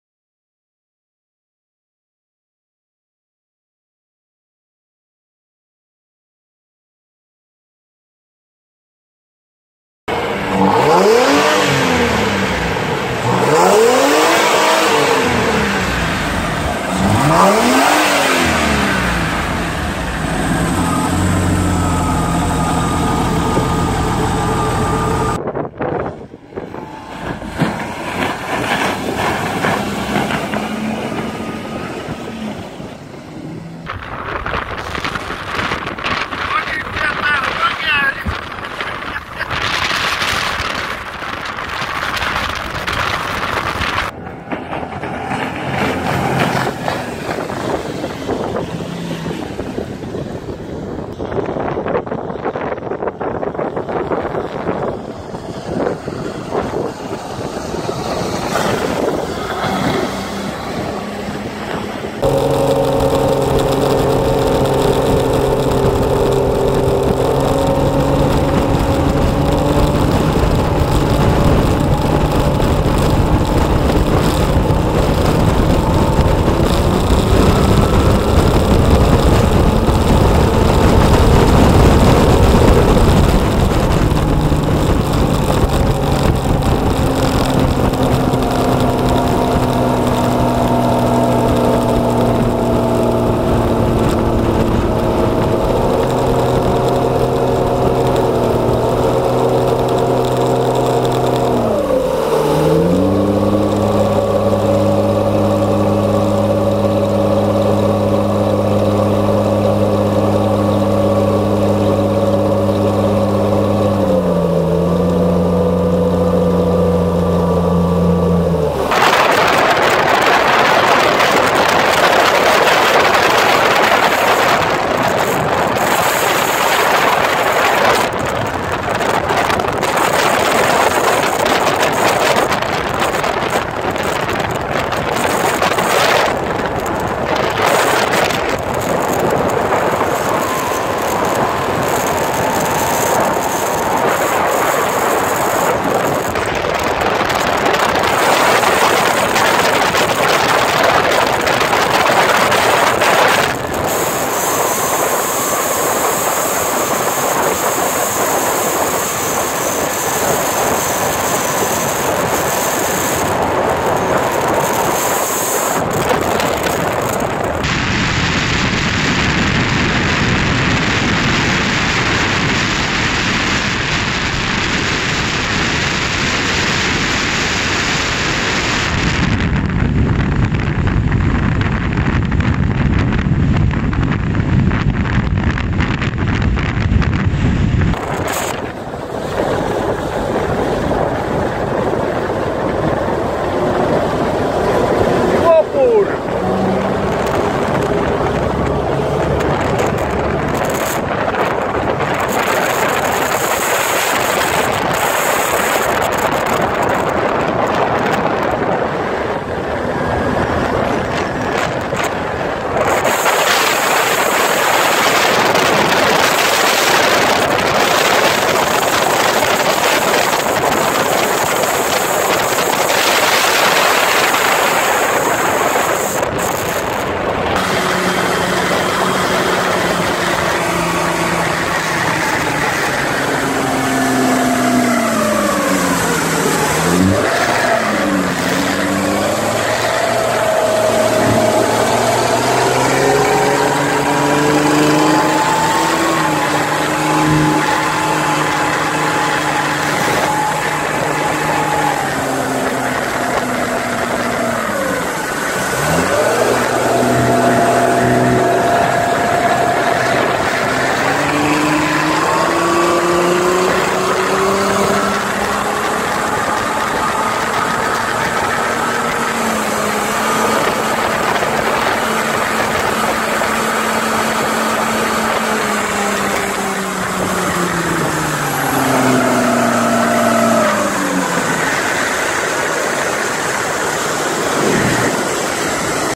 Motorówka z silnikiem AUDI 2.8 AAH tak pływa ta maszyna zbiornik wodny "NIELISZ"
Tak pływa moja maszyna, pokaz osiągów, ciąganie na kole itp. zapraszam do oglądania.